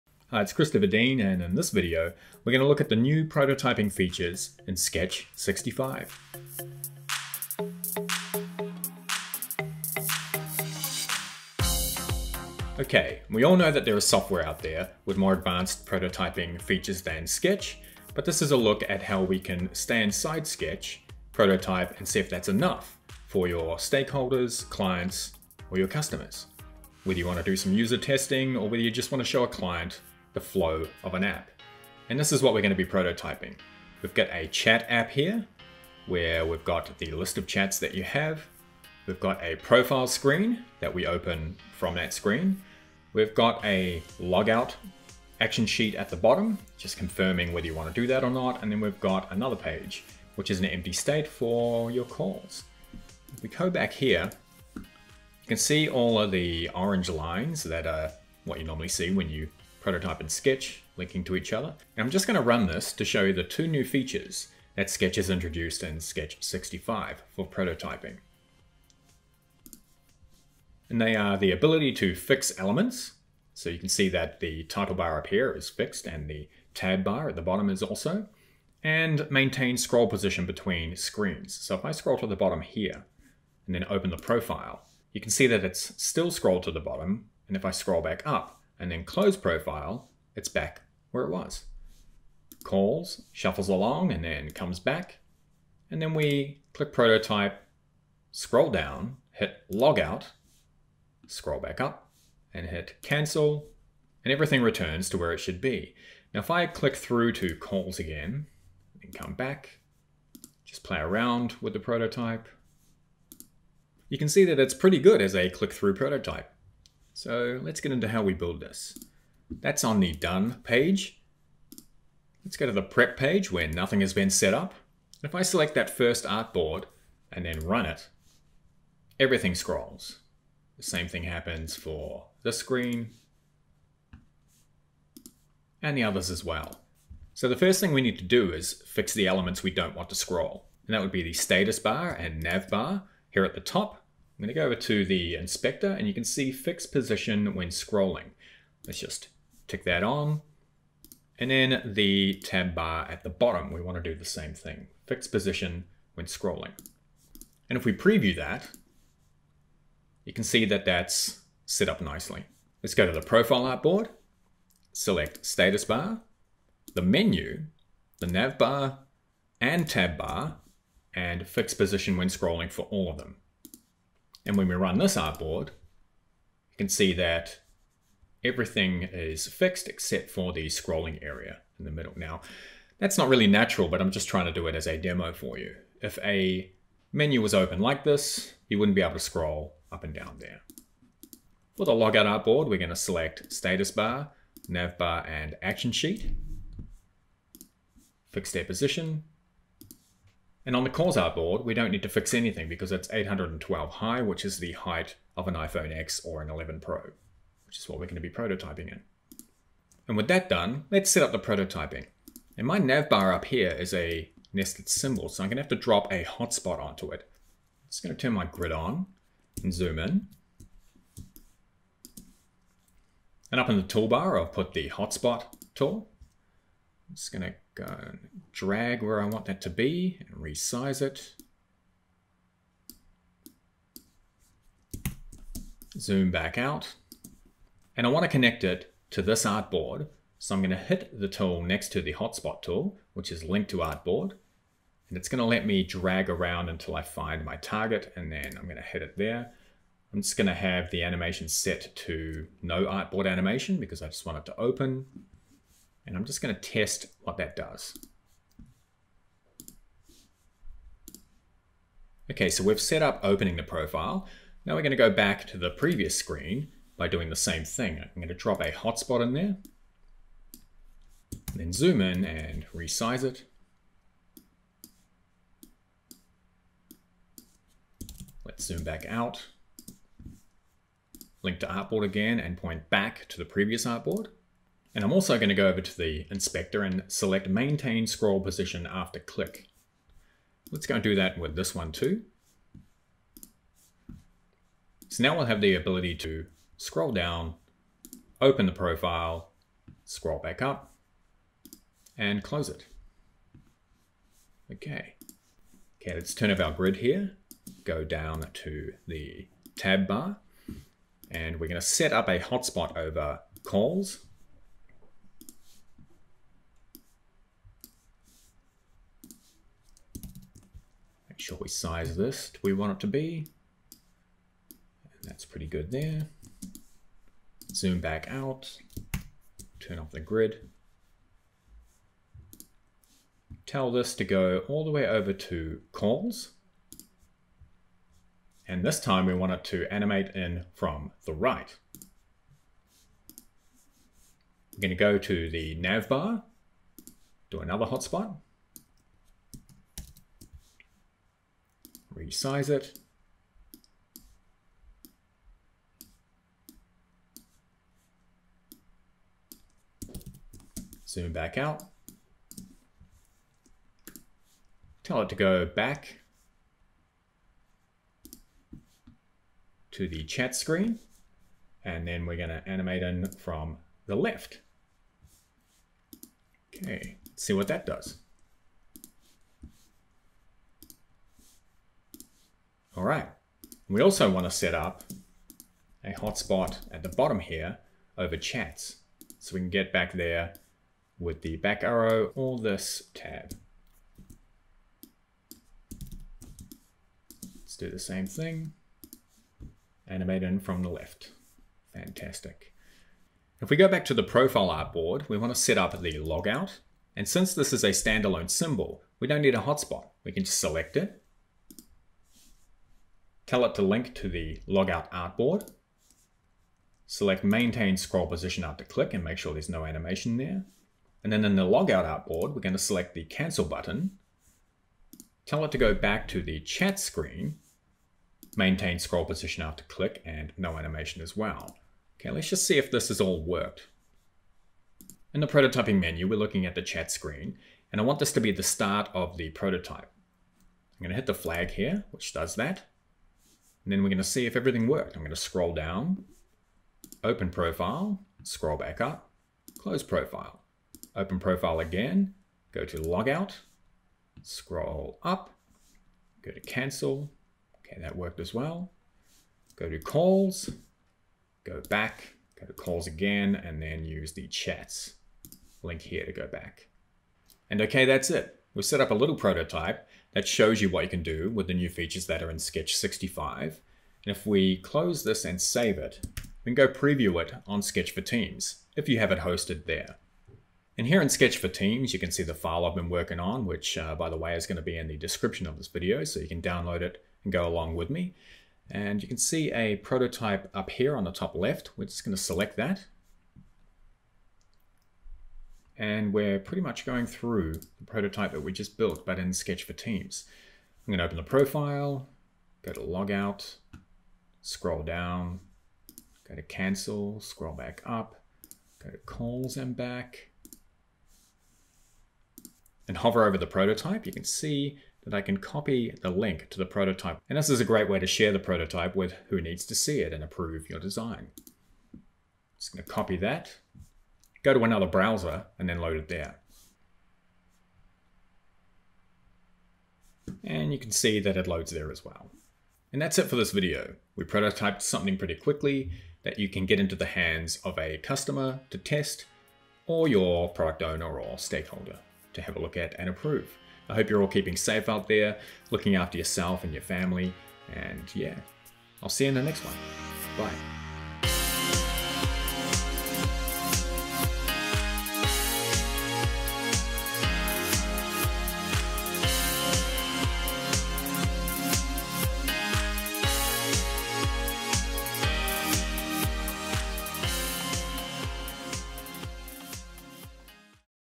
Sketch 65 Prototyping Updates
Let’s take a look at the new prototyping features inside Sketch 65 like “Fix position when scrolling” and “Maintain scroll position after click”.